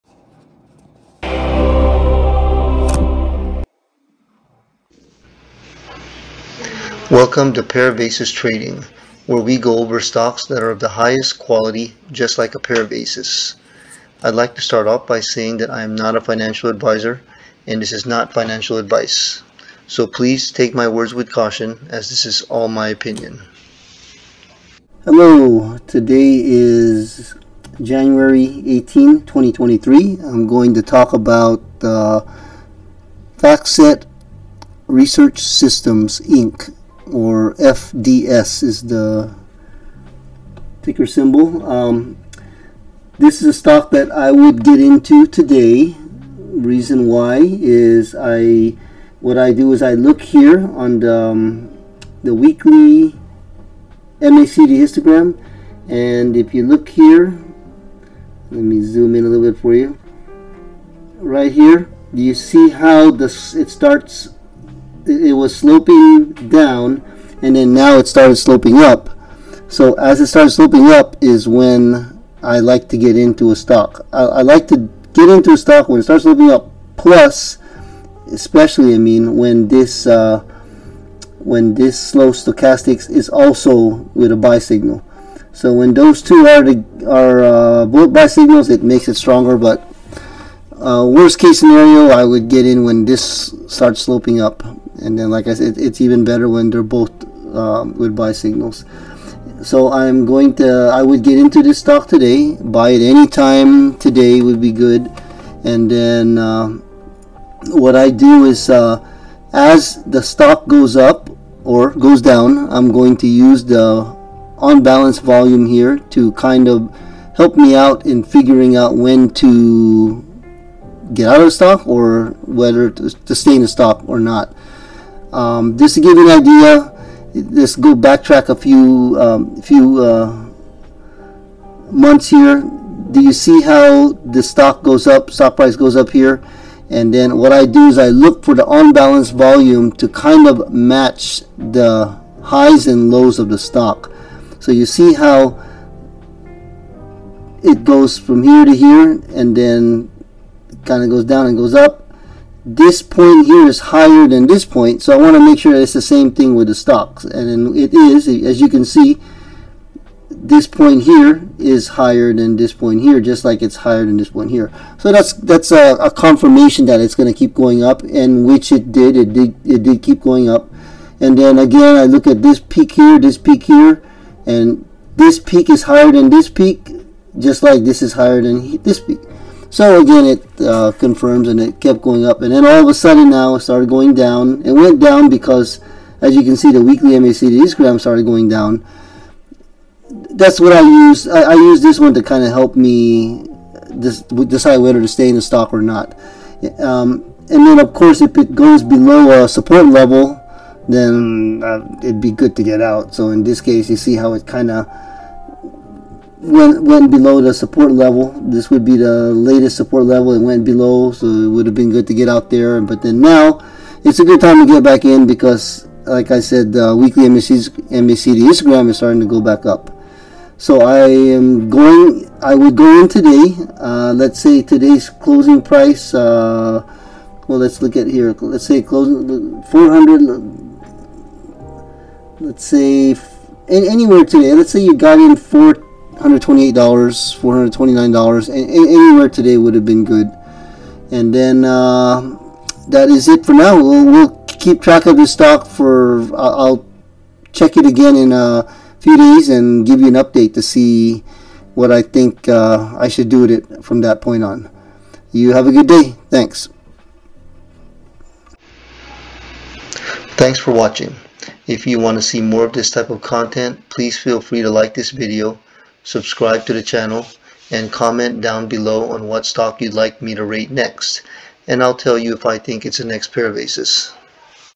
Pair Of Aces Trading - Factset Research Systems Inc (FDS) | January 18, 2023
We are back! Thank you for your patience for another long awaited video!

Today we are talking about Factset Research Systems Inc (FDS)

Open: $429.52
High: $437.19
Low: $429.99
Close: $429.42

Indicators:
- Weekly MACD
- OBV
- Slow Stochastic

Please note, we are not currently in this stock. We are analyzing it as if we did get in around $429-$437 on 01/18/2023.